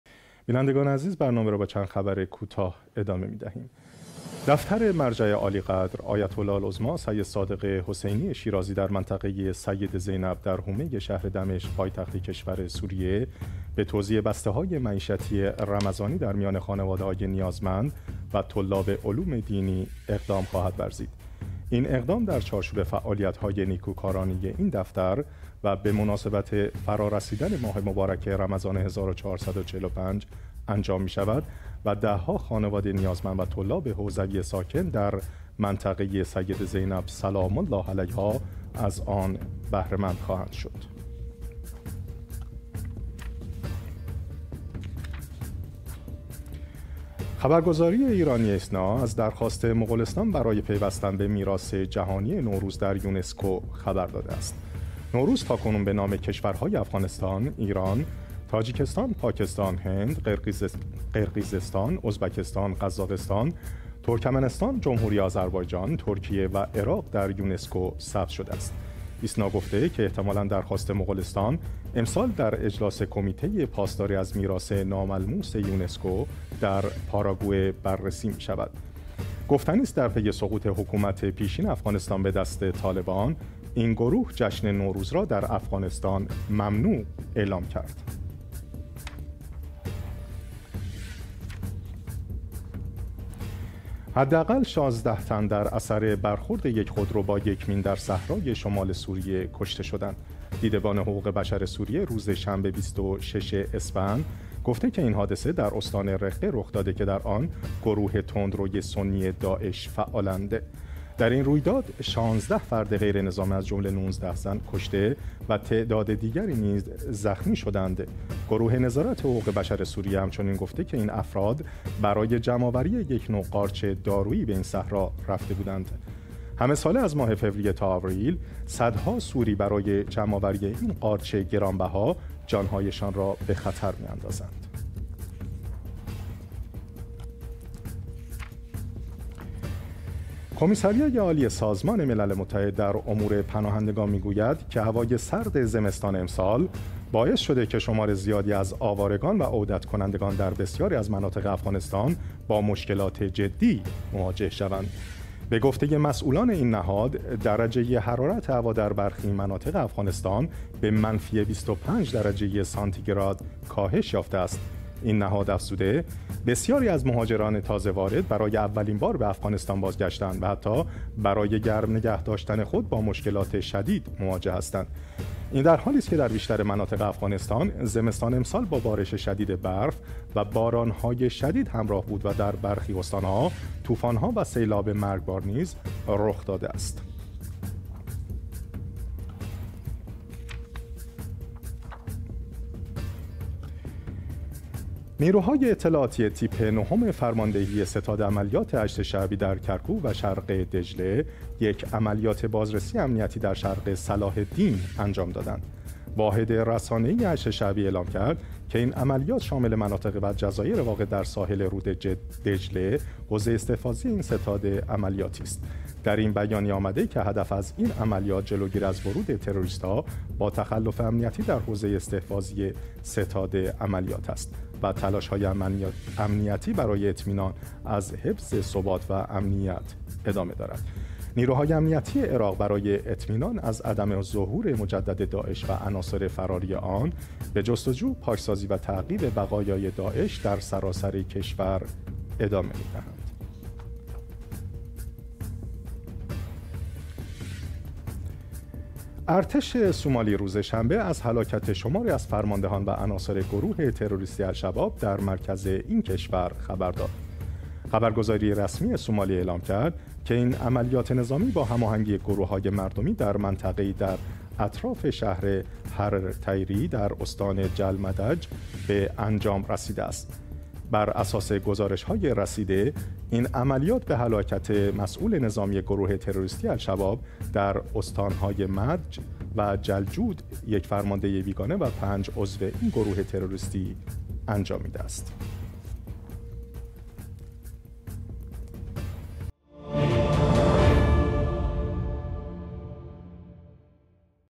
نگاهی به چند خبر کوتاه: یکشنبه 27 اسفند ماه 1402
1) توزیع بسته ها معیشتی رمضانی در منطقه سیده زینب سوریه
2)مغولستان خواستار پیوستن به میراث جهانی نوروز در یونسکو شد
3) بر اثر برخورد یک خودرو با مین در سوریه 16 تن جان باختند
4) موج سرما در افغانستان؛ سازمان ملل: خانواده های زیادی با مشکلات جدی رو به رو هستند
5) عملیات امنیتی الحشد الشعبی در شرق صلاح الدین
6هلاکت فرماندهان و عناصر گروه تروریستی «الشباب»